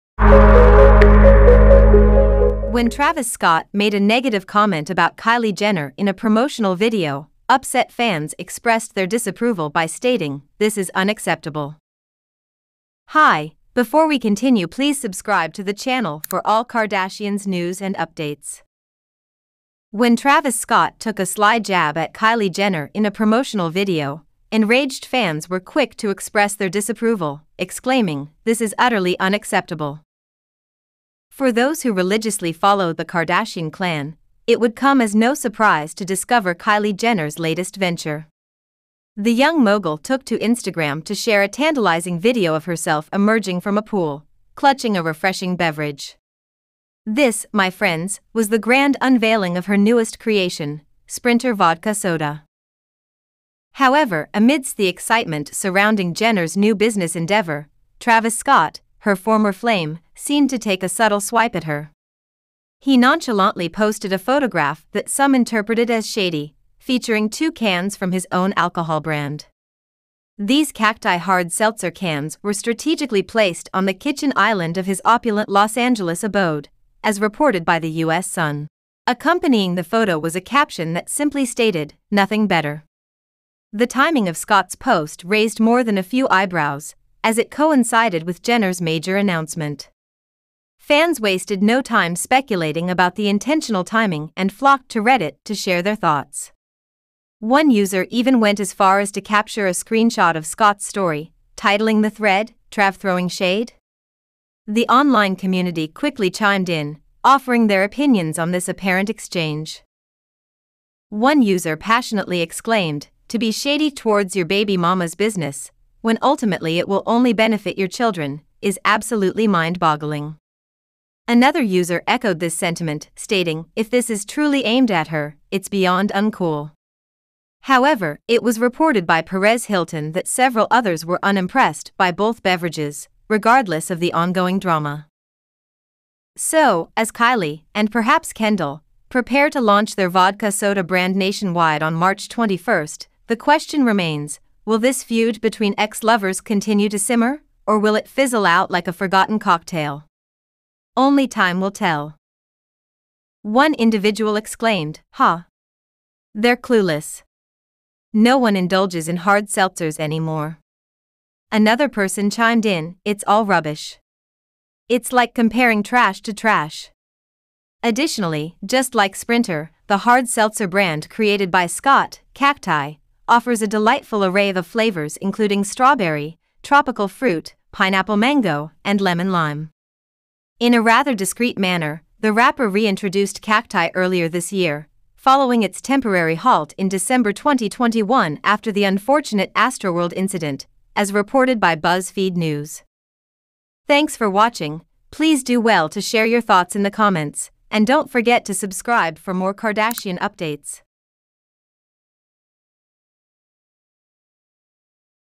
KYLIE FINISHED! Kylie Jenner Pregnant For Travis Scott And Travis PUBLISHES Her Secrets PUBLICLY
When Travis Scott made a negative comment about Kylie Jenner in a promotional video, upset fans expressed their disapproval by stating, "This is unacceptable."

When Travis Scott took a sly jab at Kylie Jenner in a promotional video, enraged fans were quick to express their disapproval, exclaiming, "This is utterly unacceptable!"

For those who religiously follow the Kardashian clan, it would come as no surprise to discover Kylie Jenner's latest venture. The young mogul took to Instagram to share a tantalizing video of herself emerging from a pool, clutching a refreshing beverage. This, my friends, was the grand unveiling of her newest creation: Sprinter Vodka Soda.

However, amidst the excitement surrounding Jenner's new business endeavor, Travis Scott, her former flame, seemed to take a subtle swipe at her. He nonchalantly posted a photograph that some interpreted as shady, featuring two cans from his own alcohol brand. These Cacti Hard Seltzer cans were strategically placed on the kitchen island of his opulent Los Angeles abode, as reported by The US Sun.

Accompanying the photo was a caption that simply stated, "Nothing better." The timing of Scott's post raised more than a few eyebrows, as it coincided with Jenner's major announcement. Fans wasted no time speculating about the intentional timing and flocked to Reddit to share their thoughts. One user even went as far as to capture a screenshot of Scott's Story, titling the thread, "Trav throwing shade?" The online community quickly chimed in, offering their opinions on this apparent exchange.